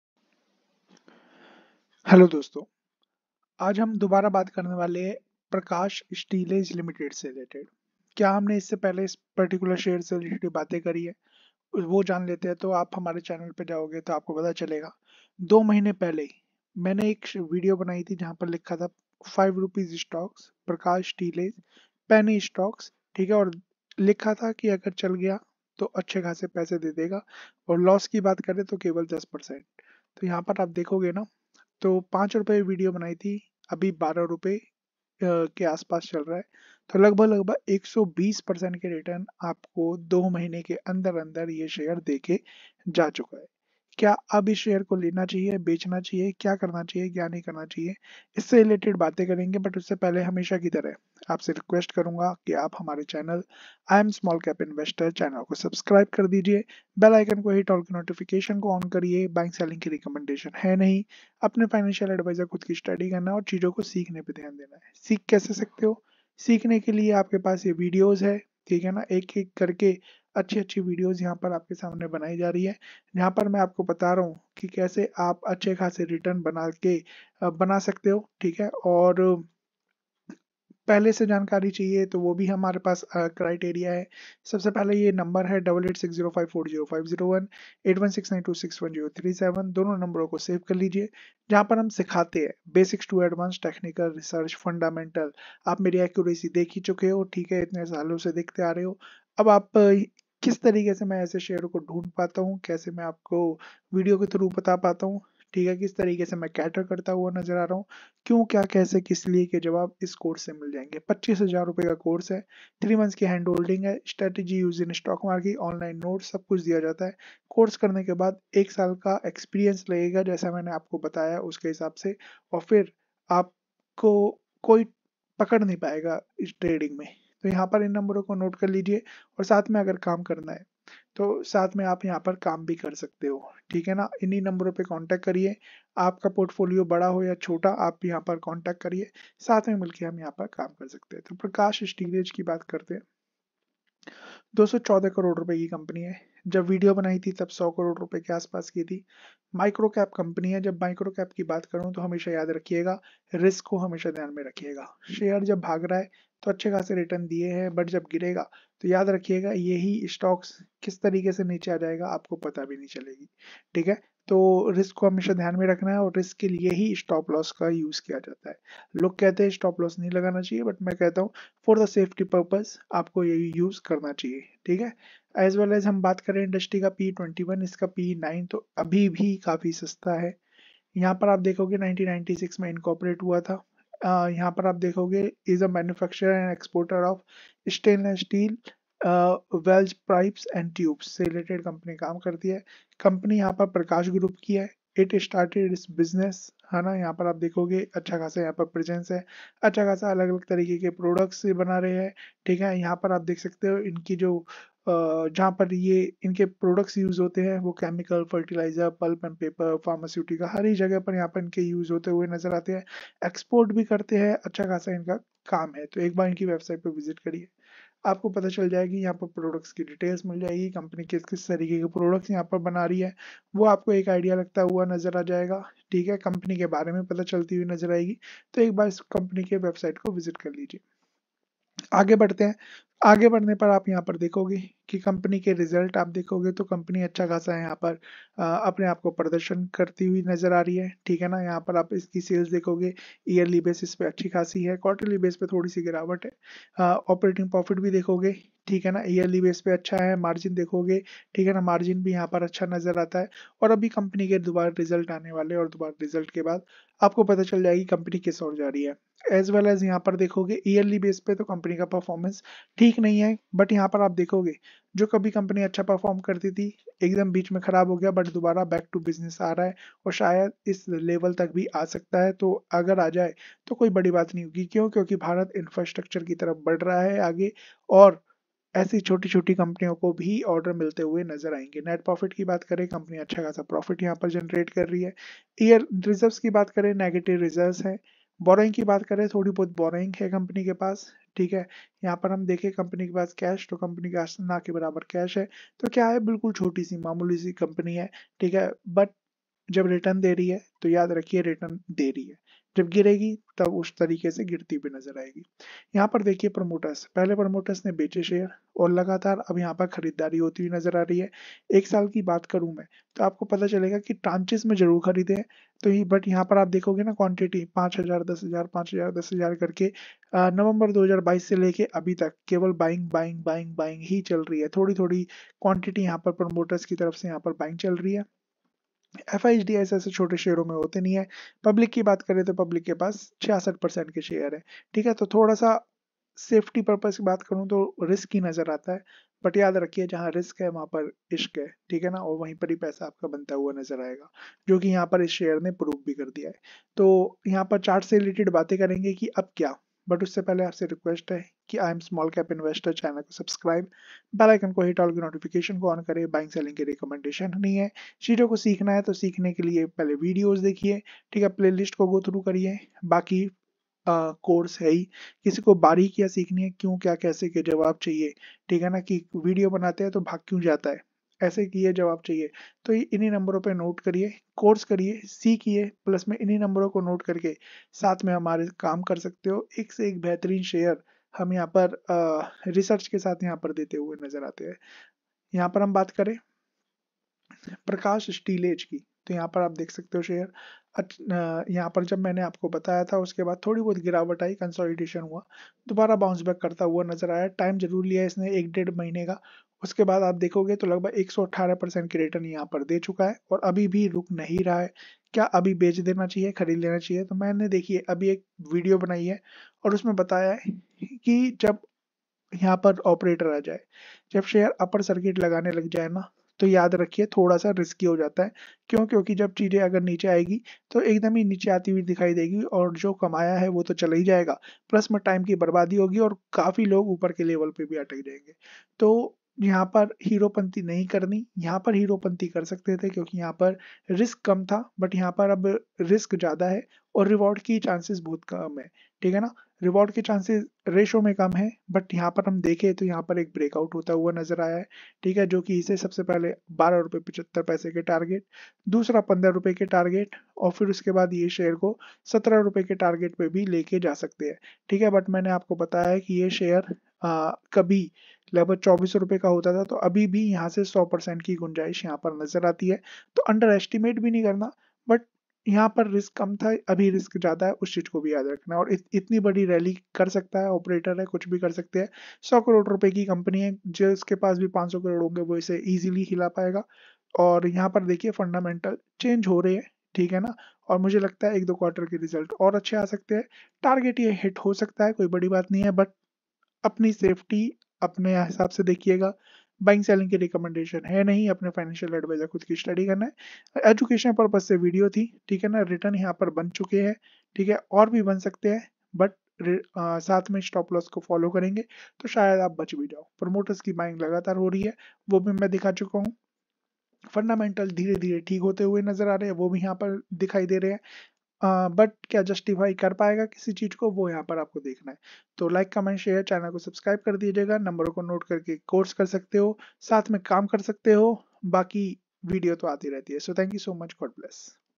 2 महीने में 120% की तेजी 🚀WITH PROOF🔥 || Prakash Steelage Ltd 🎄|| 12 RS MULTIBAGGER MICROCAP SHARE💲🚀
HI,

Prakash Industries GROUP STOCK CAN GIVE YOU A MULTIBAGGER RETURN? || WATCH THE VIDEO TO KNOW MORE?